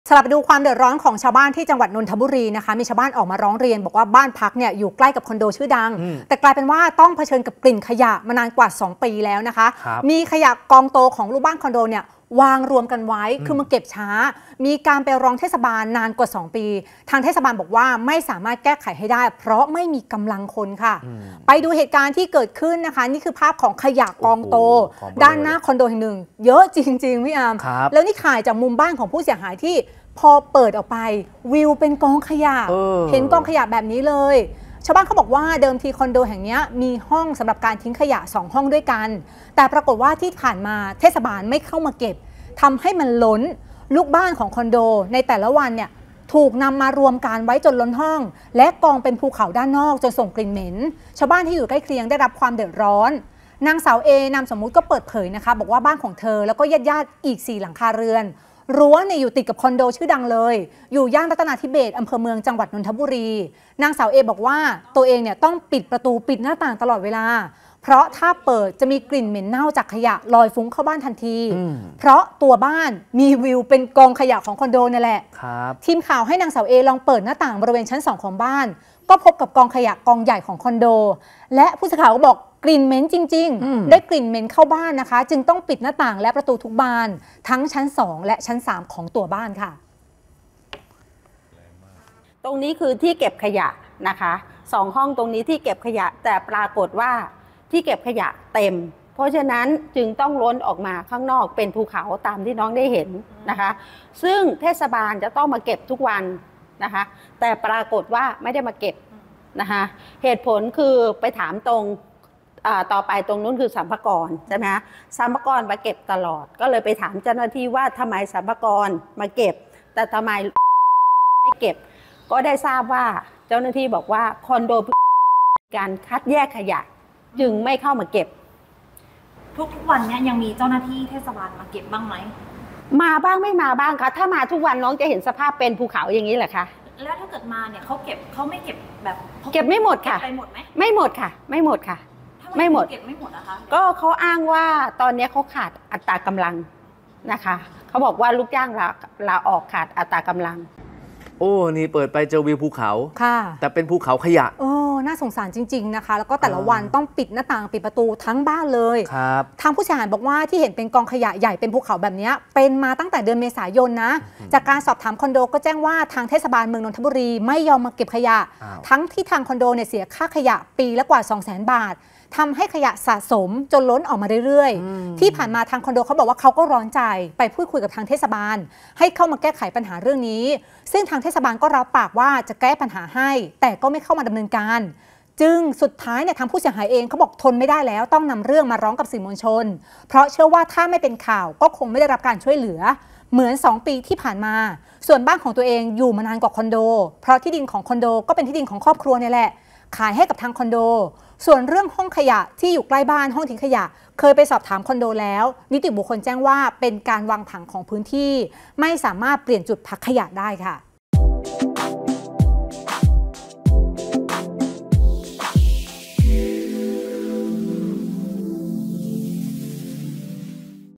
ชาวบ้านสุดทนร้องสื่อขยะกองโต ของคอนโดส่งกลิ่นเหม็น 2 ปี | เที่ยงทันข่าว | 24 มิ.ย. 67
ชาวบ้านสุดทนร้องสื่อขยะกองโต ของคอนโดส่งกลิ่นเหม็น เปิดบ้านไม่ได้ ร้องเทศบาล 2 ปี ไม่ดำเนินการให้ เทศบาลอ้างขาดกำลังคน